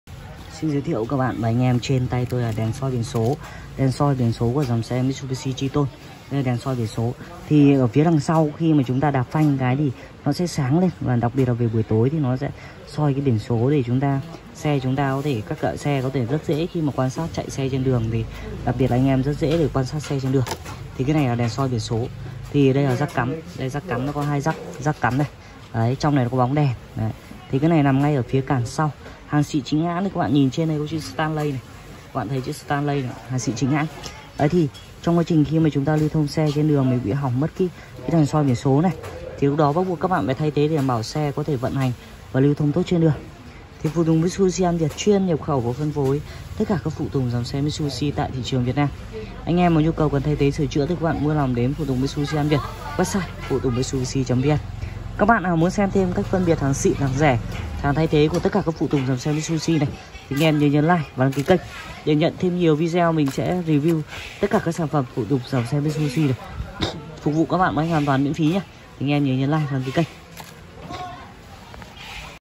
Đèn soi biển số xe mitsubishi Triton 2016-2019
Đèn soi biển số xe Mitsubishi Triton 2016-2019 là chi tiết phụ tùng rất quan trọng của mỗi chiếc xe. Khi lưu thông trên đường vào buổi tối hoặc ban đêm, Đèn soi biển số có nhiệm vụ sáng lên, giúp anh em phía sau dễ dàng quan sát và giữ khoảng cách an toàn.